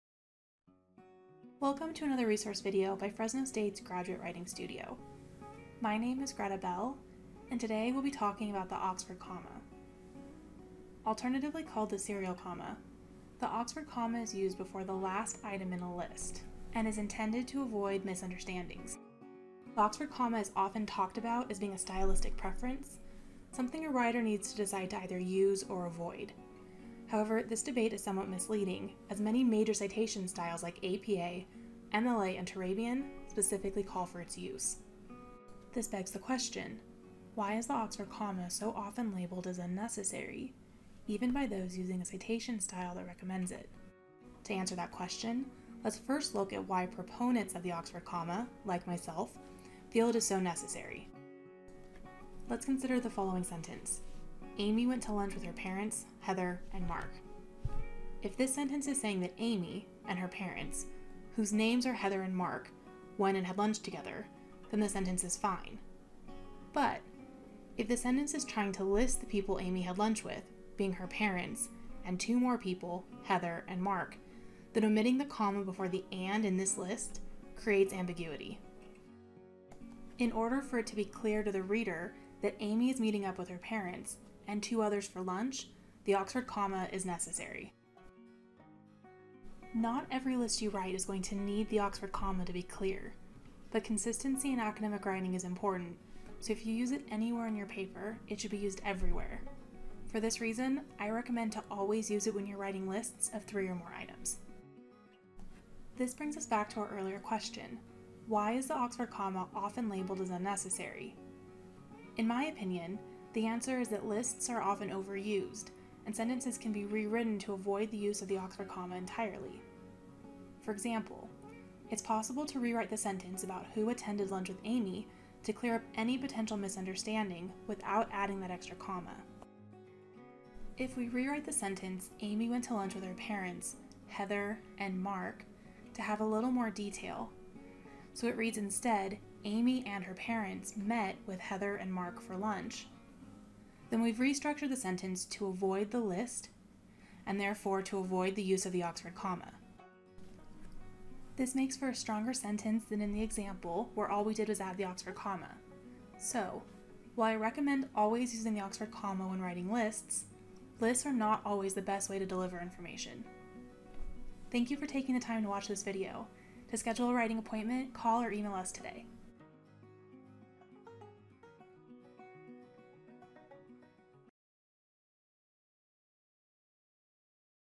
Oxford Comma
Are you using the Oxford comma? Should you be? Check out this video to find out!